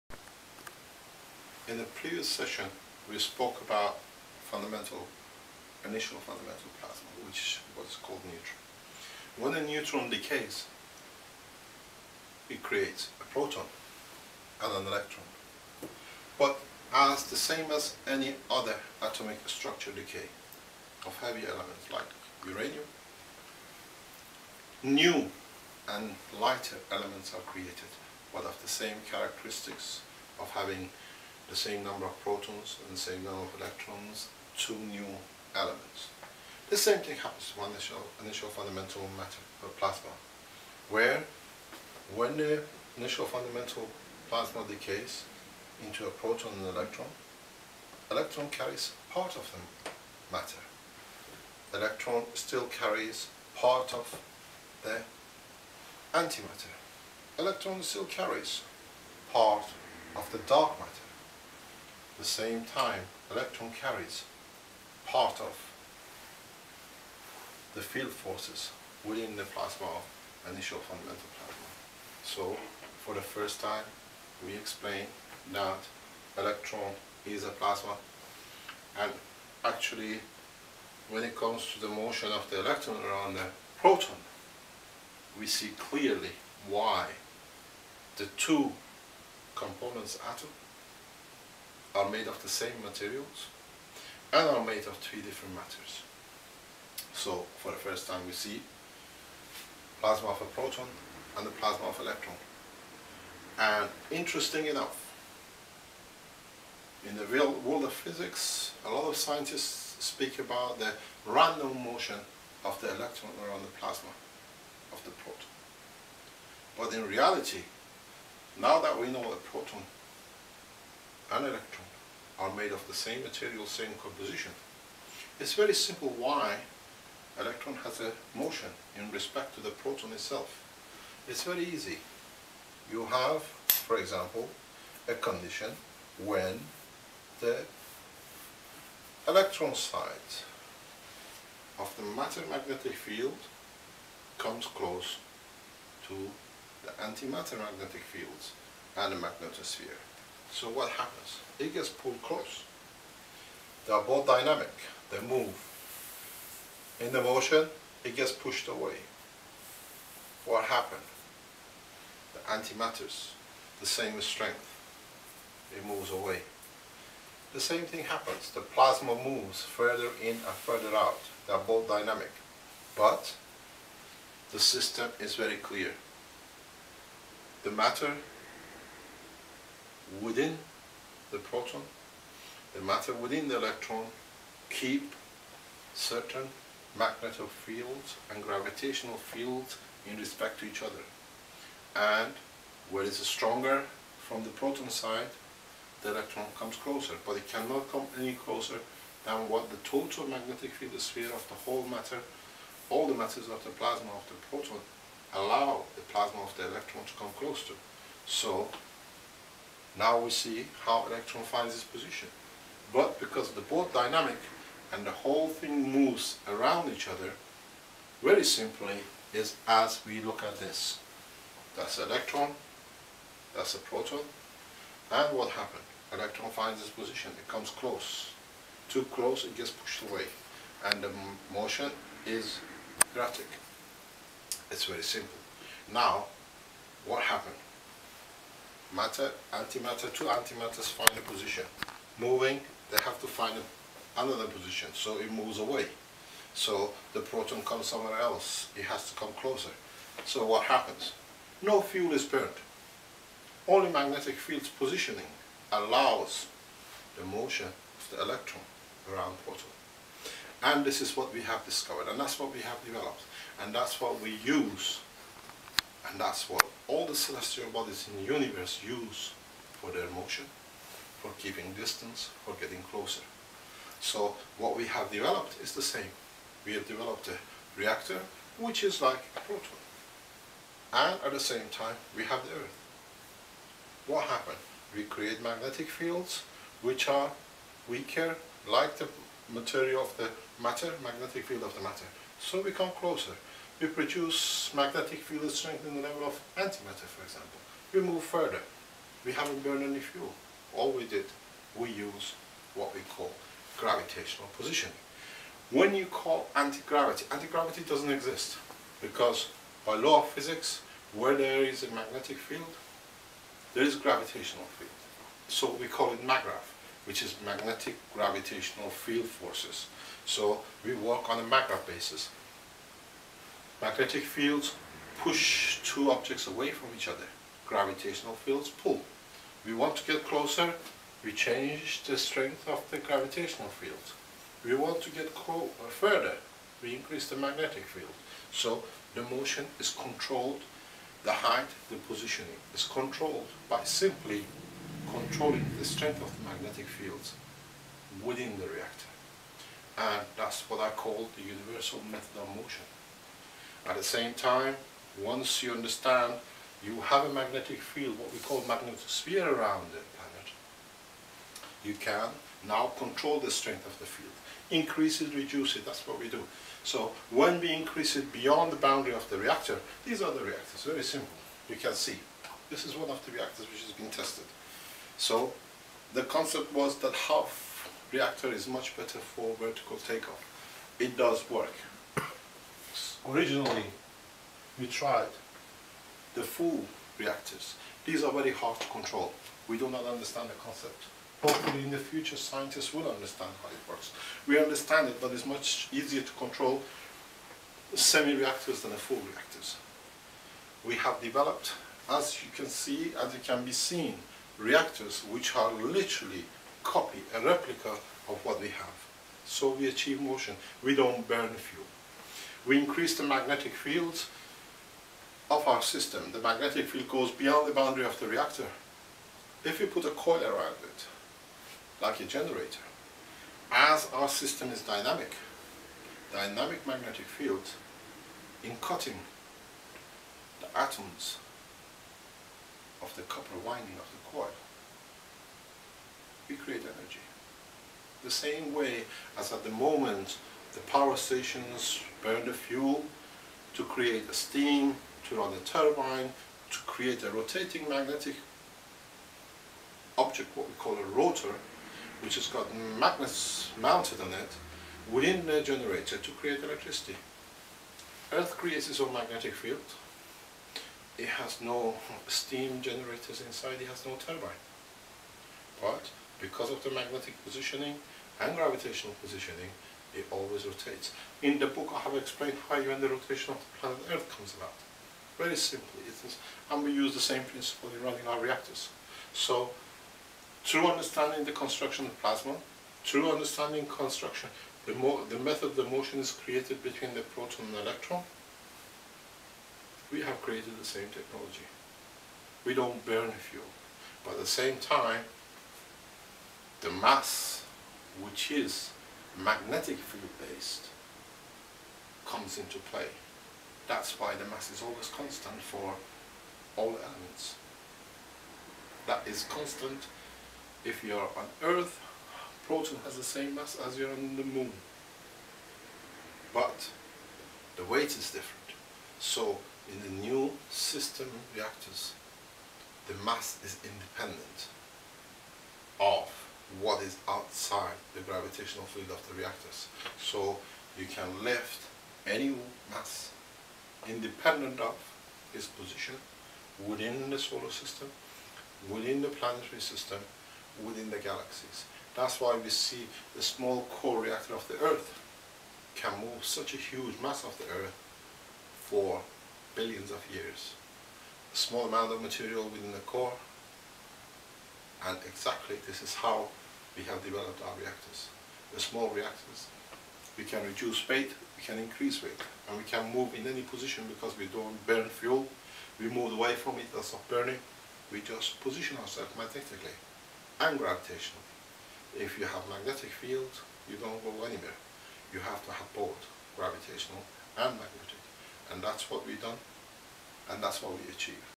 Keshe 3 Nuclear decay and Motion
In this third movie about his new book, titled: The Universal Order of Creation of Matters, nuclear engineer Mehran Keshe shows the new and more logic way to understand the natural nuclear decay of the neutron into a proton, an electron and some smaller fragments of energy (like light, gamma rays, etc.). Tiny magnetic fields are the real fundamental particles originating from a cosmic magnetic soup. Fields of similar or equal strength can entangle in a dynamic pack. Three of such packs with different strength make the three basic type of Matters (Antimatter, Dark Matter and Matter), and they can interlock to make the Initial fundamental Plasma or Neutron. This neutron can decay into a plasma of a proton and of a plasma of an electron, and both still contain parts of the Matters trinity of the original plasma. They have each an individual, and together an overall, magnetospheric condition which is ruled by Magnetic and gravitational fields strengths. This is similar to the inward Gravitational and outward Magnetic fields of Earth. Based on these new first principles prototype reactors were build, with amazing results, like creating lift and motion without burning fuel, and achieve fusion in a simple way. Now it becomes possible to create at any point-of-demand in an affordable way vast amounts of energies (like electricity and heat) and motion, and matters like water, food and new materials, and to develop new space technology.